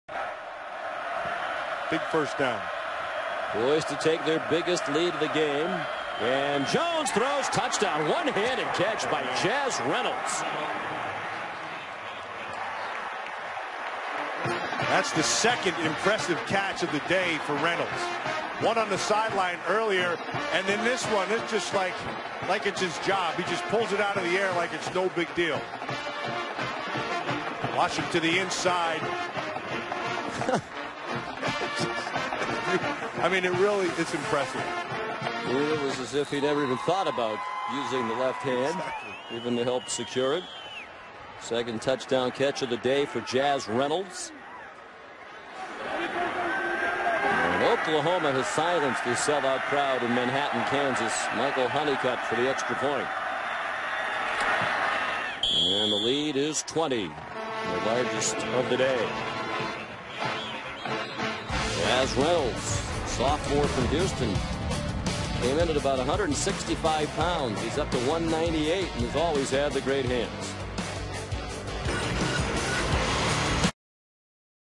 OU Jaz Reynolds 1 handed TD Reception vs. KSU 2011
Jaz Reynolds great one-handed Catch vs. Kansas St. Oct 29, 2011

Property of ESPN